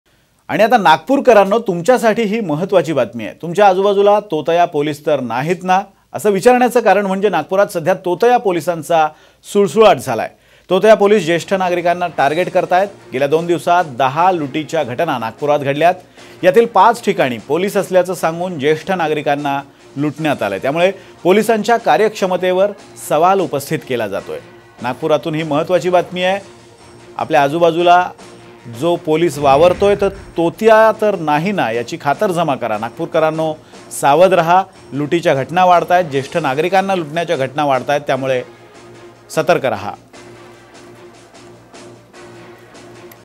Nagpur : 24 तासांत नागपुरात 10 ठिकाणी लुटीच्या घटना, पाहा सविस्तर बातमी | SAAM TV
Watch Latest News Updates on Nagpur : 24 तासांत नागपुरात 10 ठिकाणी लुटीच्या घटना, पाहा सविस्तर बातमी